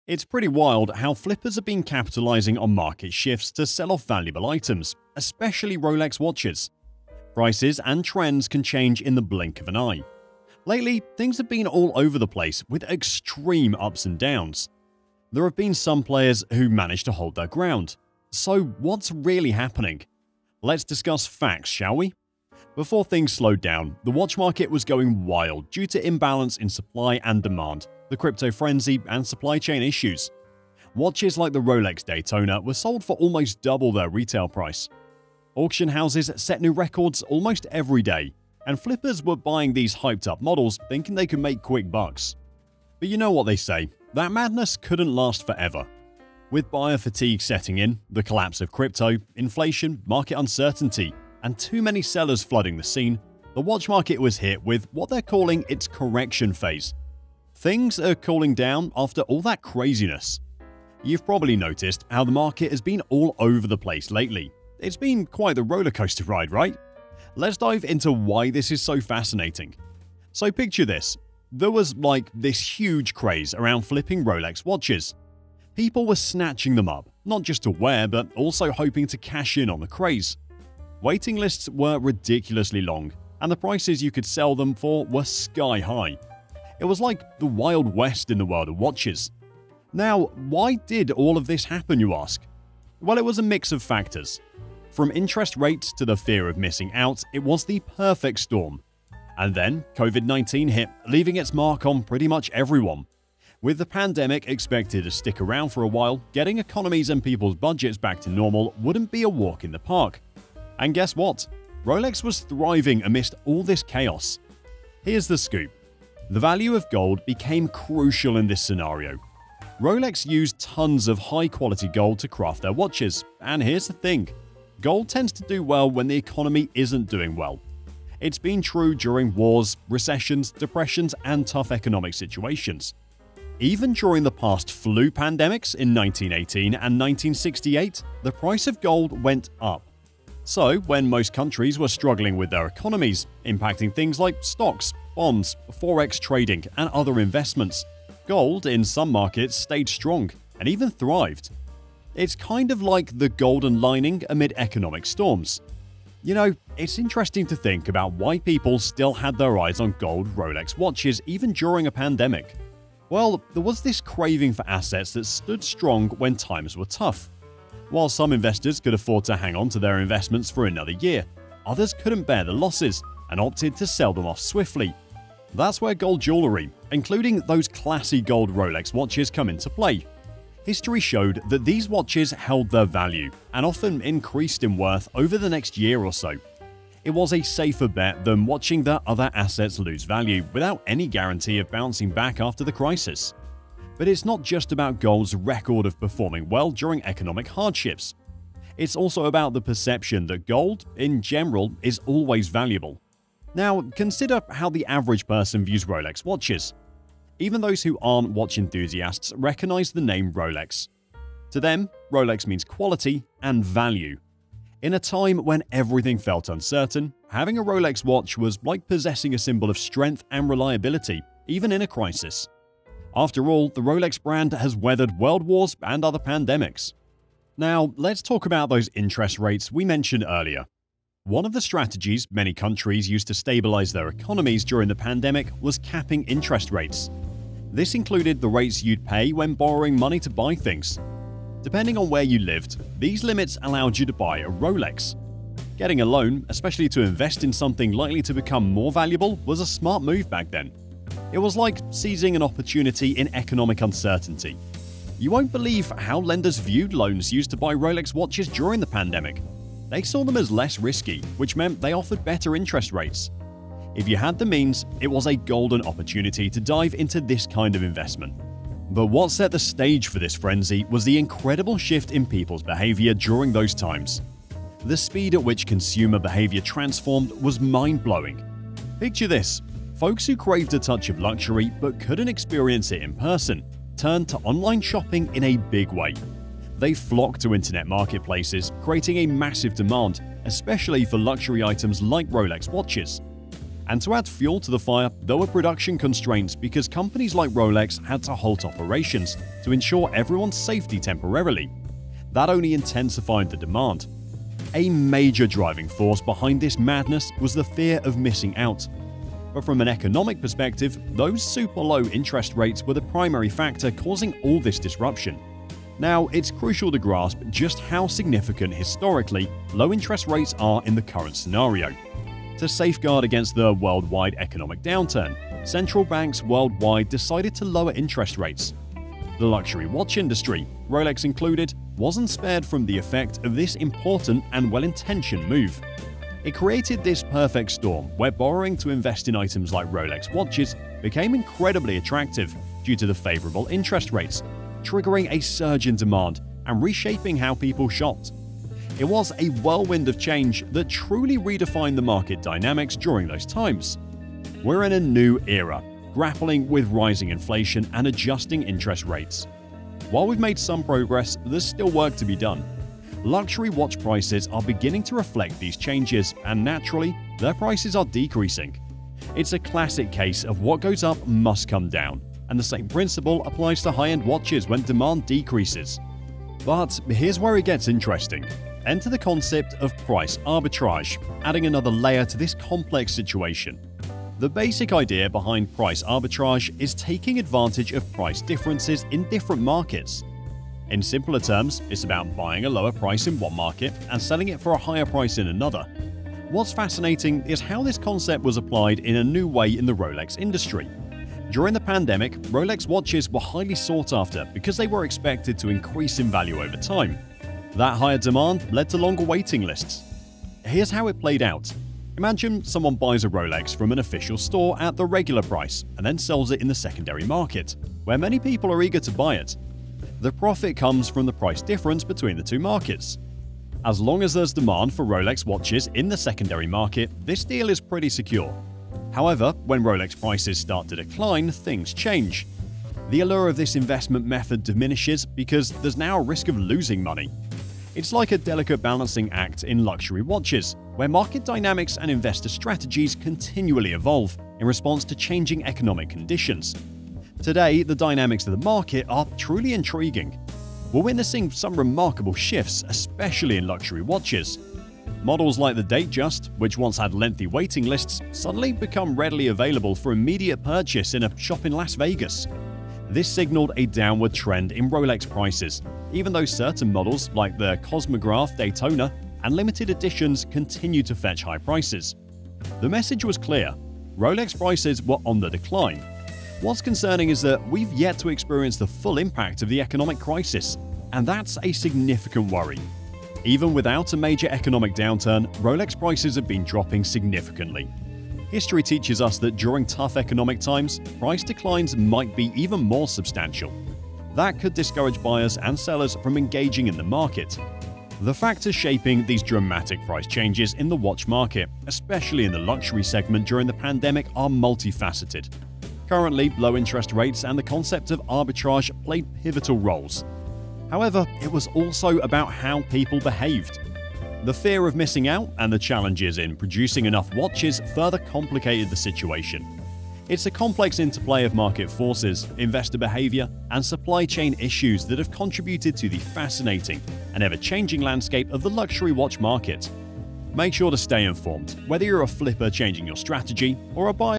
Is the Rolex Flipping Era OVER?
It's pretty wild how flippers have been capitalizing on market shifts to sell off valuable items, especially Rolex watches. Prices and trends can change in the blink of an eye. Lately, things have been all over the place, with extreme ups and downs. There have been some players who managed to hold their ground. So, what's really happening? Let’s discuss facts, shall we?
Body
Before things slowed down, the watch market was going wild due to imbalances in supply and demand, the Crypto frenzy, and supply chain issues. Watches like the Rolex Daytona were sold for almost double their retail price. Auction houses set new records almost every day, and flippers were buying these hyped-up models, thinking they could make quick bucks.

But you know what they say, that madness couldn't last forever. With buyer fatigue setting in, the collapse of Crypto, inflation, market uncertainty, and too many sellers flooding the scene, the watch market has hit what they're calling its 'Correction Phase.' Things are cooling down after all that craziness.

You've probably noticed how the market has been all over the place lately – it's been quite the rollercoaster ride, right? Let's dive into why this is so fascinating. So, picture this: there was this huge craze around flipping Rolex watches. People were snatching them up, not just to wear but also hoping to cash in on the craze. Waiting lists were ridiculously long, and the prices you could sell them for were sky-high. It was like the Wild West in the world of watches.

Now, why did all of this happen, you ask? Well, it was a mix of factors. From low-interest rates to the fear of missing out, it was like the perfect storm. And then, COVID-19 hit, leaving its mark on pretty much everyone. With the pandemic expected to stick around for a while, getting economies and people's budgets back to normal wouldn't be a walk in the park. But guess what? Rolex was thriving amidst all this chaos.

Here's the scoop: the value of gold became crucial in this scenario. Rolex used tons of high-quality gold to craft their watches, and here's the thing – gold tends to do well when the economy isn't doing so well. It's been true during wars, recessions, depressions, and tough economic situations. Even during past flu pandemics in 1918 and 1968, the price of gold went up. So, when most countries were struggling with their economies, impacting things like stocks, bonds, Forex trading, and other investments, gold, in some markets, stayed strong and even thrived. It's kind of like the golden lining amid economic storms.